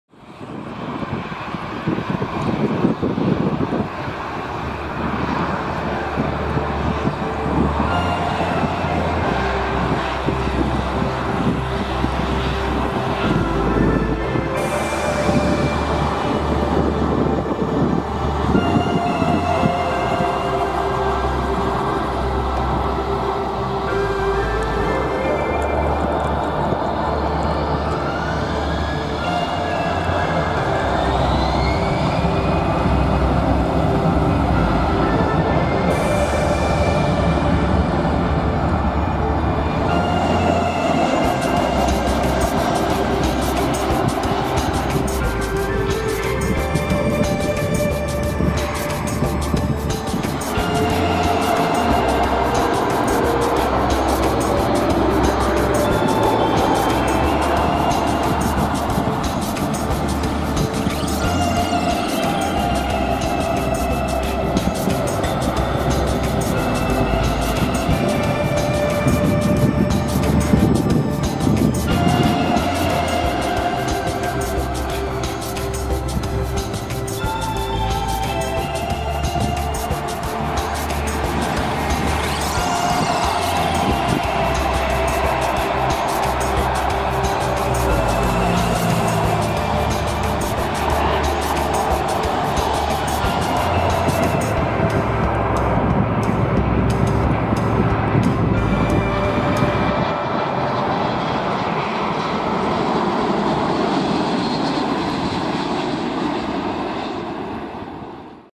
Landings and Take Offs Airbus & Boeing | Plane Music Video
Music video compilation with landings and take offs. Planes included are, Boeing 737, Boeing 777-300er, Airbus A330-300, A330-900neo, A320, A310.

Ryanair Boeing 737-8AS - 0:03
TAP Portugal Stopover Livery Airbus A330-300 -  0:17
Aer Lingus Airbus A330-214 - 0:26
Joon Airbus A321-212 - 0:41
Turkish Airlines Airbus A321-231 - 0:52
Emirates Boeing 777-300 ER - 1:00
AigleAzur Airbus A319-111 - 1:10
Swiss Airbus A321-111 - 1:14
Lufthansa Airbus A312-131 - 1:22
TAP Airbus A330-900 Neo - 1:35